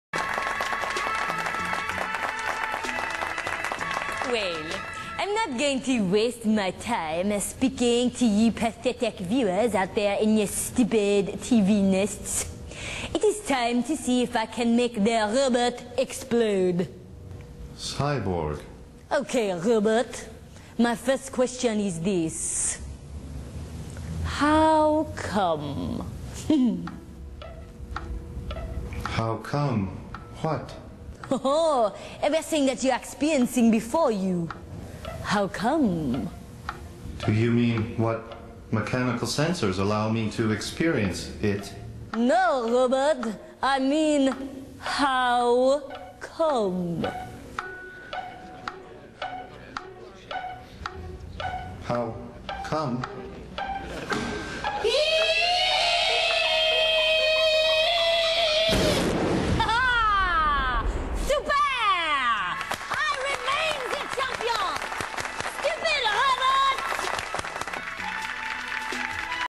how come?
a robot used to detect supercool is asked the age old question.